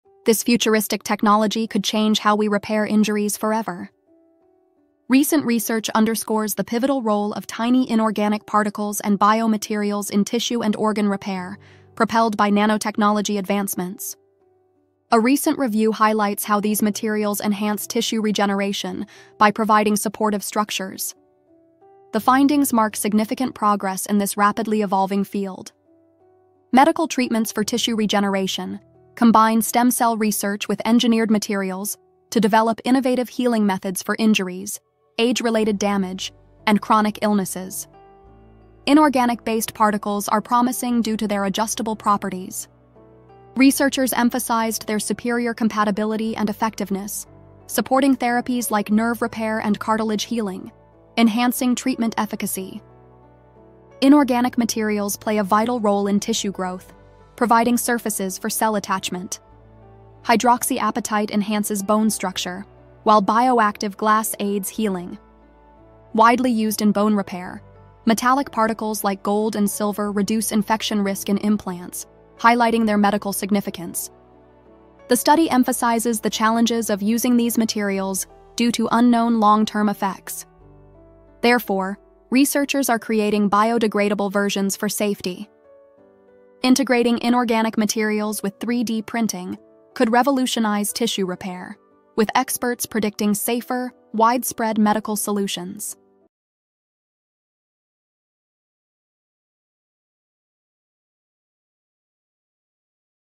This futuristic technology could change how we repair injuries forever!
Revolutionizing Injury Repair with Nanotechnology 💡🦾

Futuristic medical treatments are closer than ever! A new study published in Inorganics highlights how inorganic particles and advanced biomaterials are transforming tissue repair and regeneration. 🌱🔬

Led by Dr. Nabanita Saikia from New Mexico Highlands University, the research reveals that nanomaterials like hydroxyapatite, bioactive glass, and metallic nanoparticles can significantly boost cell growth, enhance bone repair, and even fight infections in medical implants.

The fusion of nanotechnology and biomedicine could soon revolutionize injury repair, making faster recovery and better outcomes a reality.